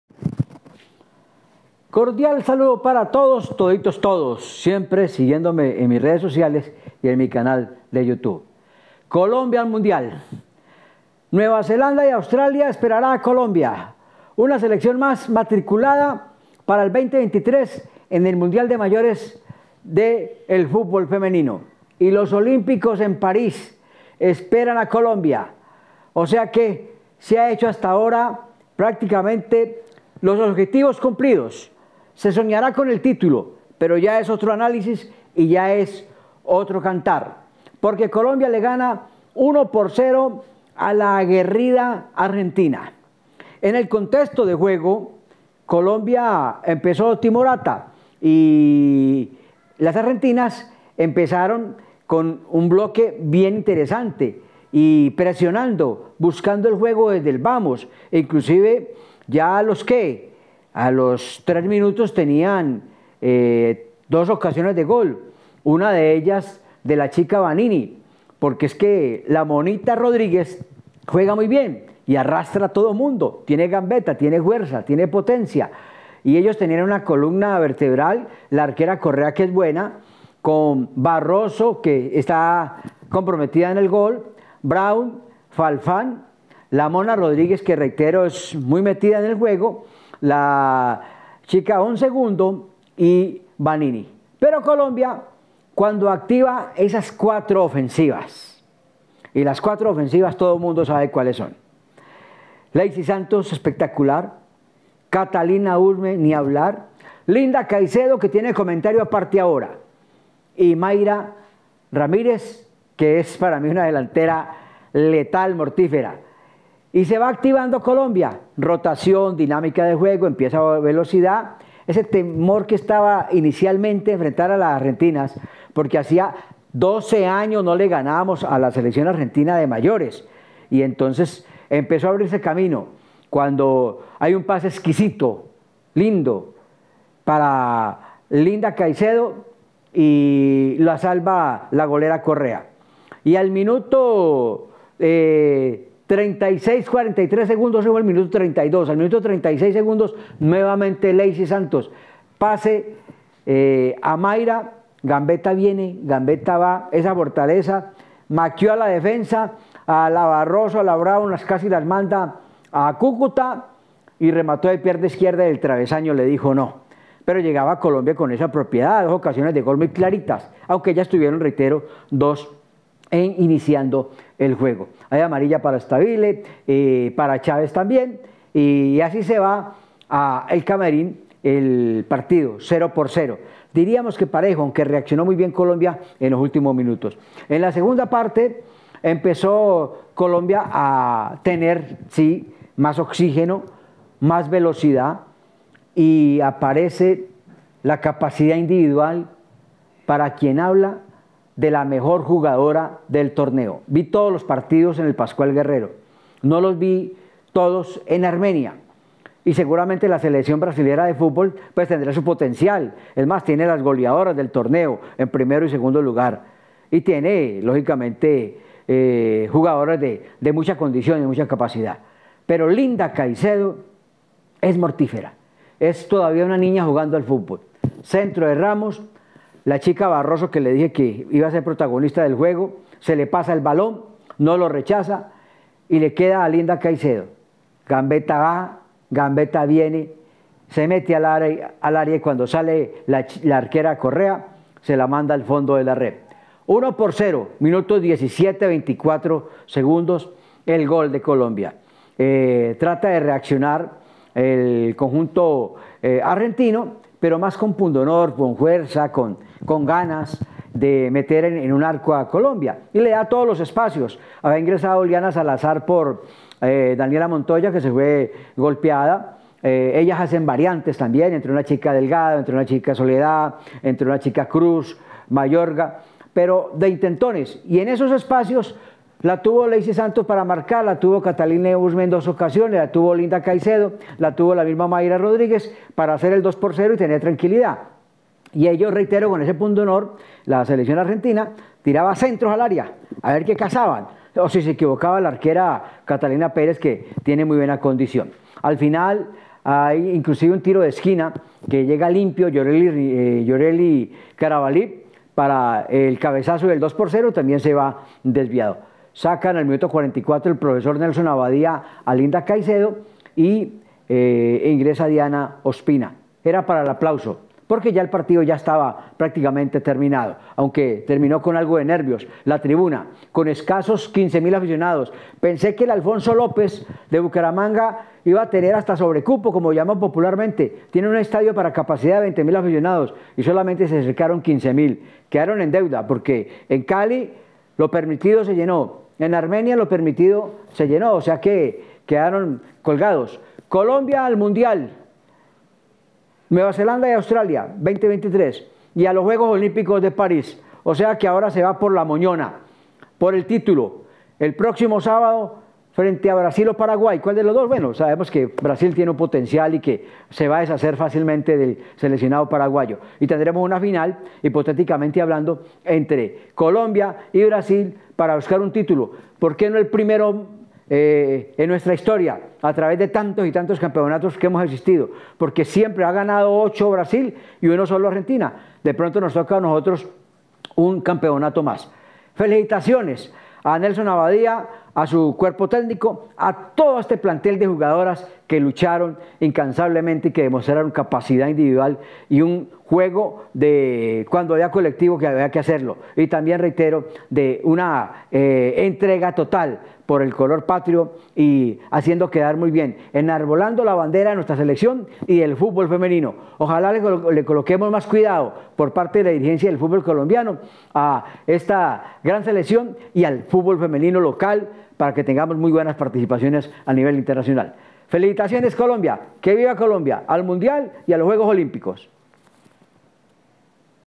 ¿ Y QUÉ DIRÁ CALCETO ? De la clasificación de Colombia al Mundial y olímpicos. 1x0 vs Argentina.
¿ Y QUÉ DIRÁ CALCETO ? De la clasificación de Colombia al Mundial y Olímpicos. 1x0 vs Argentina. La capacidad individual de Linda Caicedo extraordinario, la mejor del torneo.